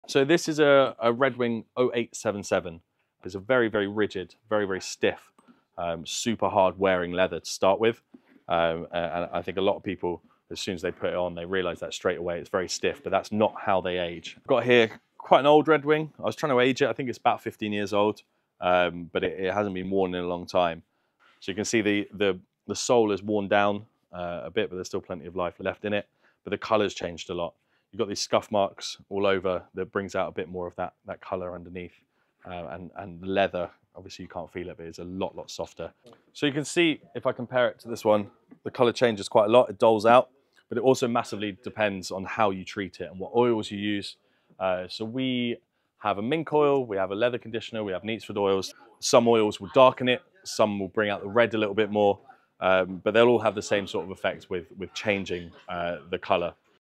How do Red Wing Boots age over time?
PART 1 of our Better with Age series of shorts. These Red Wing 0877 boots age differently depending on how you care for them. Sam goes over how the leather and sole holds up over time, comparing a brand new pair with ones which have been worn for a while!